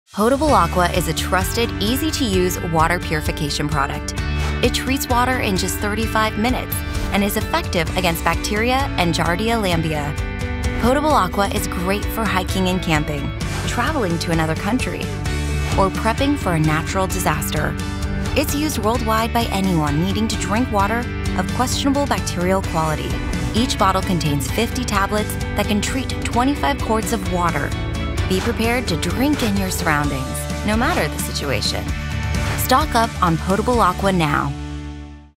Potable Aqua® Water Purification Tablets
Safe Drinking Water. No matter the source.

The most trusted name in water purification products, Potable Aqua® is used by militaries and relief organizations around the world. Potable Aqua® products provide time-tested, simple-to-use water treatment options for campers, backpackers, hikers, international travelers and other outdoor enthusiasts.